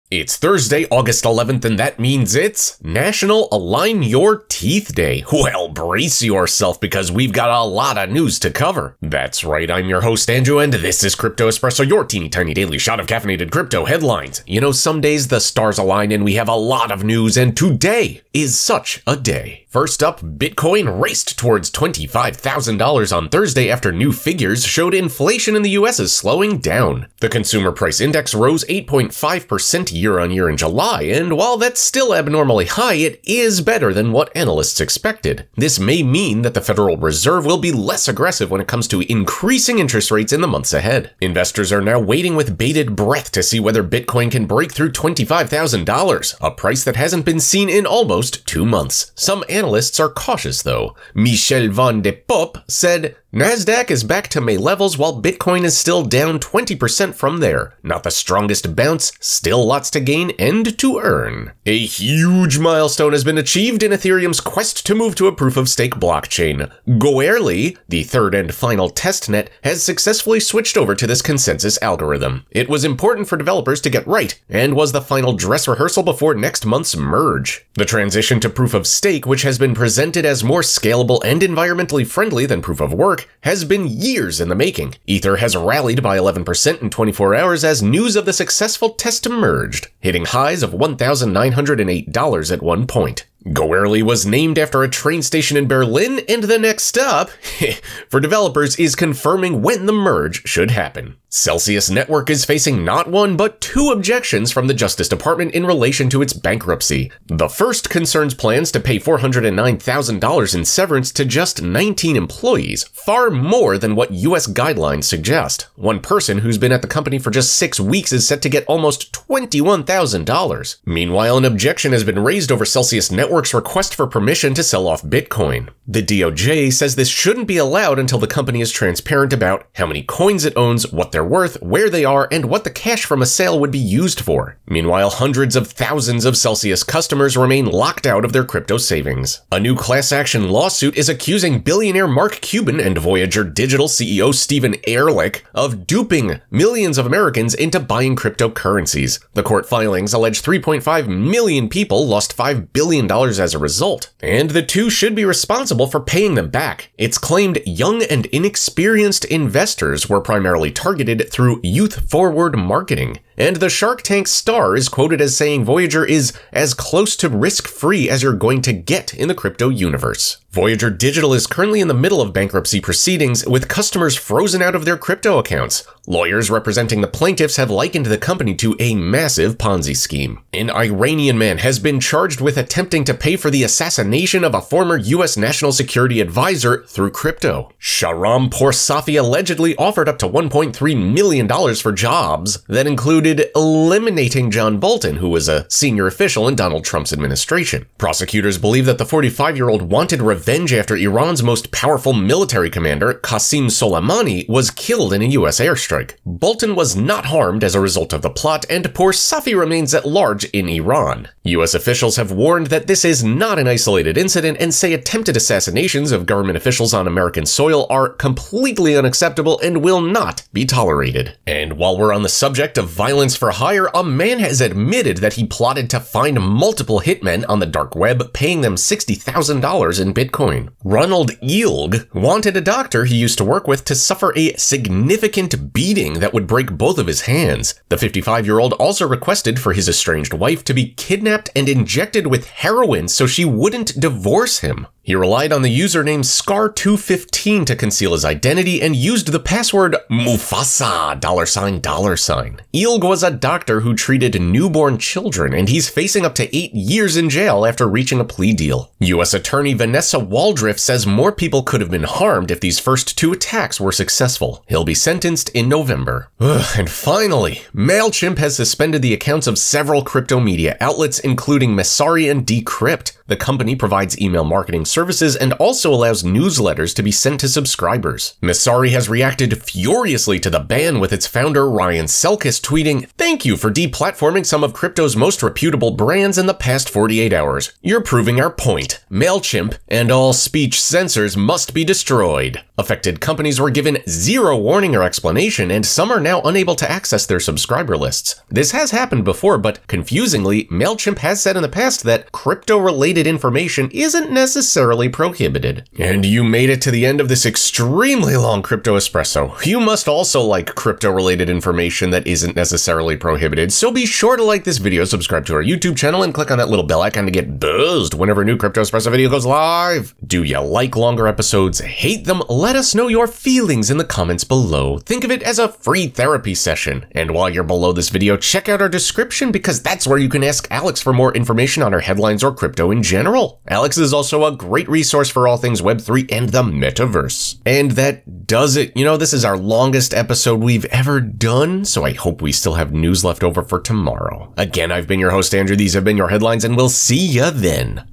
Ethereum on Brink of Biggest Upgrade in Its History [Crypto Espresso 11.08.22]
For more information about crypto and today's stories, just Ask Alex

🔵 CoinMarketCap Daily:

• Coinbase and BlackRock to Give Institutional Investors Greater Access to Crypto

"Our institutional clients are increasingly interested in gaining exposure to digital asset markets," BlackRock says.

• Nomad Bridge Reveals Bounty It'll Pay to White Hat Hackers After $190M Exploit

The latest figures from Etherscan suggest $22 million has been sent to the official recovery address, but that's just 11.5% of the funds that are missing.

• Meta Rolling Out NFTs to 100 Countries Worldwide… But Not Europe

This is a significant milestone in bringing crypto collectibles to the masses, and for many who use Instagram, this will likely be the first time they come into contact with NFTs.

• Block's Gross Profit Surges, But Bitcoin Revenues Plunge 34%

In a letter to shareholders, Block said there's been declining consumer demand for Bitcoin in recent months.